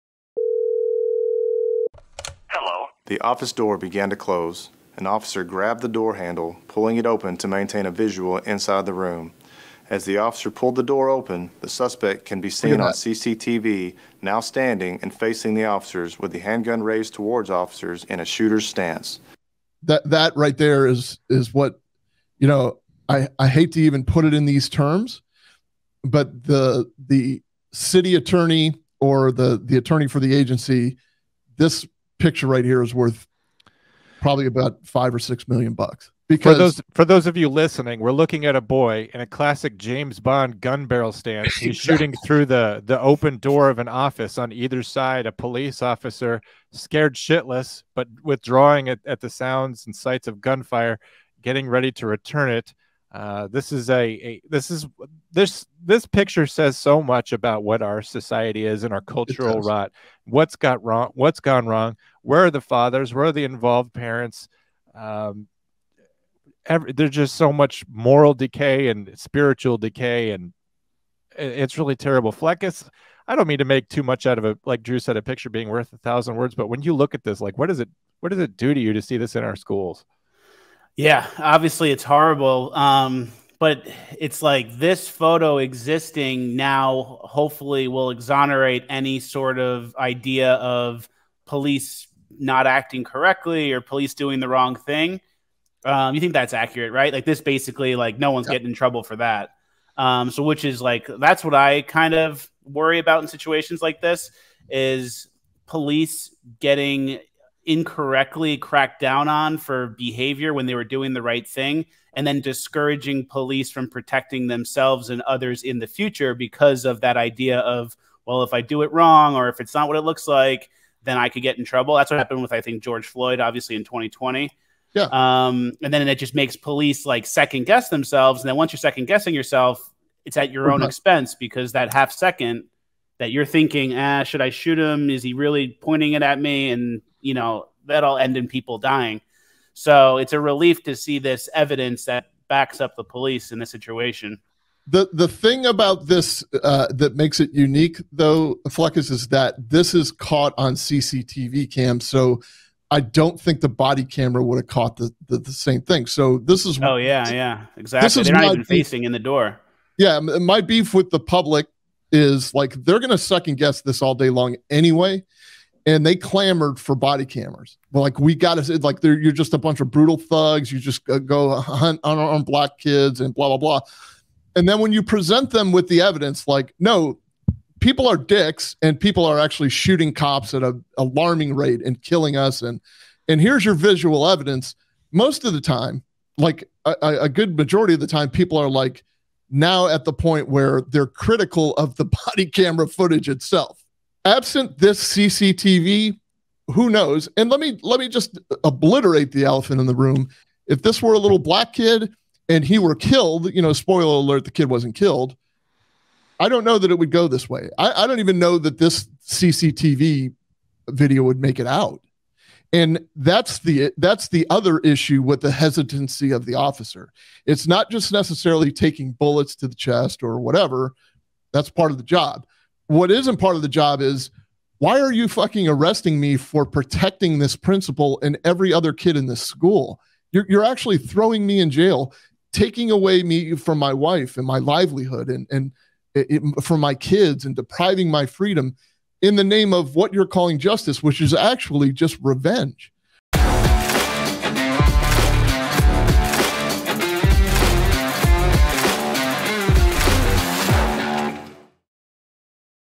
Teen Gunman Faces Off with Police: Shocking CCTV Footage Exposed!
A teen stands in a classic James Bond gun-barrel stance ready to fire at officers in Mesquite, TX. Where have we gone wrong as a society to get to this place? Drew, Jon, and guest Fleccas breakdown the police body cams and CCTV footage released from the Mesquite school shooting.

Administrators & officers attempted to talk down the student who brought a handgun into the school. The 911 call released along with bodycam video from Mesquite police, shows the administrator calling 911 and pleading with the student to put the gun down. Officers also attempted to negotiate with the student before any use of force was used.

Want to be part of the conversation? Call in and talk with the guys live OR leave a voicemail and we will play it on the show!